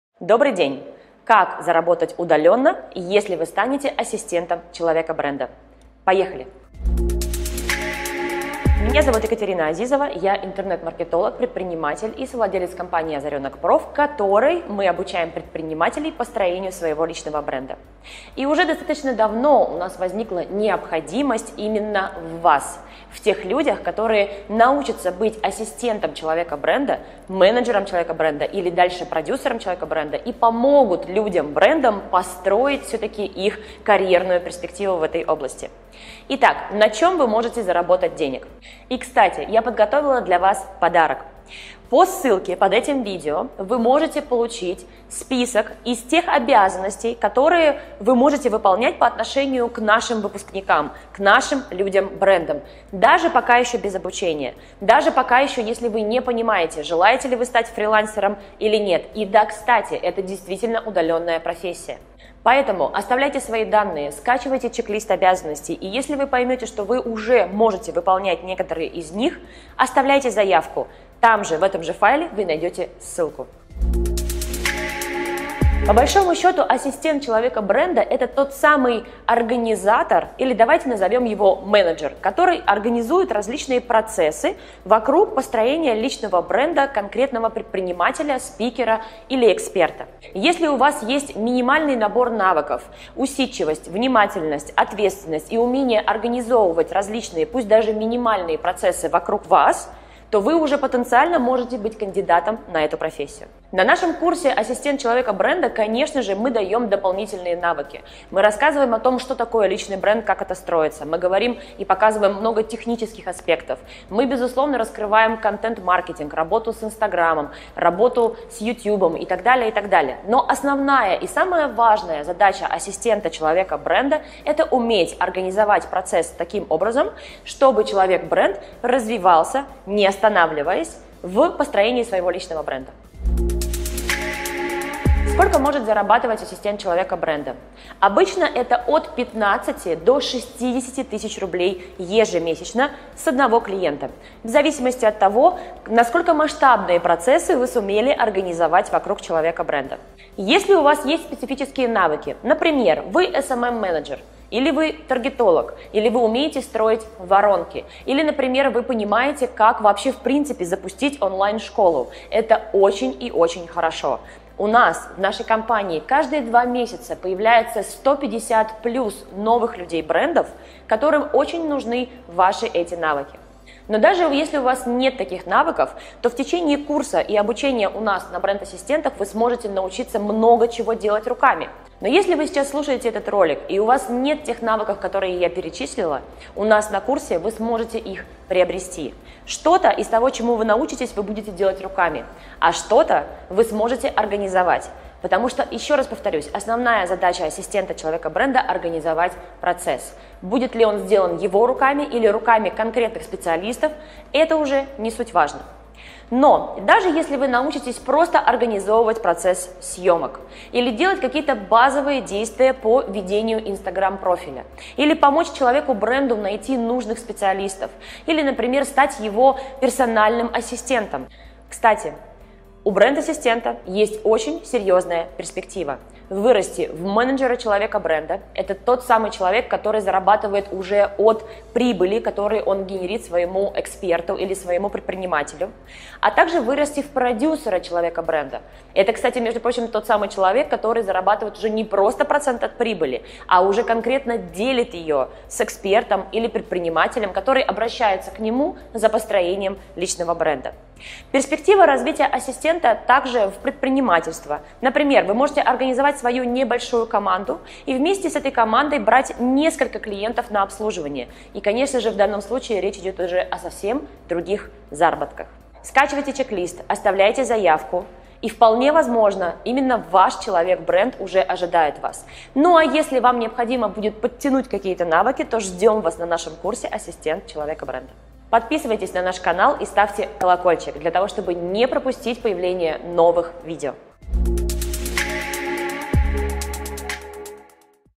КАК ЗАРАБАТЫВАТЬ ЧЕРЕЗ ИНТЕРНЕТ от 15 000 руб.? Удаленный Заработок Ассистента Человека-Бренда //16+
Интересно узнать, как зарабатывать через интернет? Сегодня мы с вами обсудим тему: “Удаленный заработок ассистента человека-бренда”.
В AzarenokPRO мы обучаем ассистентов людей-брендов, рассказывая, как зарабатывать через интернет, помогая предпринимателям развивать свой личный бренд. Из видео вы узнаете, какими минимальными навыками вам необходимо обладать, чтобы получать удаленный заработок уже сейчас.
Расскажу, как зарабатывать онлайн от 15 000 до 60 000 рублей с одного клиента, работая бренд-ассистентом, а также какие перспективы имеет данная удаленная профессия.
Если вы в поисках ответа на вопрос “Как зарабатывать через интернет?”, скачивайте список обязанностей ассистента человека-бренда и начните осваивать новую удаленную профессию уже сейчас.

00:09 Как зарабатывать через интернет?
01:18 Удаленная профессия ассистент человека-бренда
02:16 Удаленный заработок ассистента
03:42 Перспективы бренд-ассистента

Видео выходят во вторник и пятницу в 12-00 по МСК.

Как заработать удаленно, если вы станете ассистентом человека-бренда? Поехали. Меня зовут Екатерина Азизова, я интернет маркетолог, предприниматель, и совладелец компании Азаренокпро, в которой мы обучаем предпринимателей построению своего личного бренда. И уже достаточно давно у нас возникла необходимость именно в вас, в тех людях, которые научатся быть ассистентом человека-бренда, менеджером человека-бренда. Или дальше продюсером человека-бренда. И помогут людям-брендам построить все-таки их карьерную перспективу в этой области. Итак, на чем вы можете заработать денег? И кстати, я подготовила для вас подарок. По ссылке под этим видео вы можете получить список из тех обязанностей, которые вы можете выполнять по отношению к нашим выпускникам, к нашим людям–брендам. Даже пока еще без обучения. Даже если пока еще вы не понимаете, желаете ли вы стать фрилансером или нет. И да, кстати, это действительно удаленная профессия. Поэтому оставляйте свои данные, скачивайте чек лист обязанностей и если вы поймете, что вы уже можете выполнять некоторые из них – оставляйте заявку.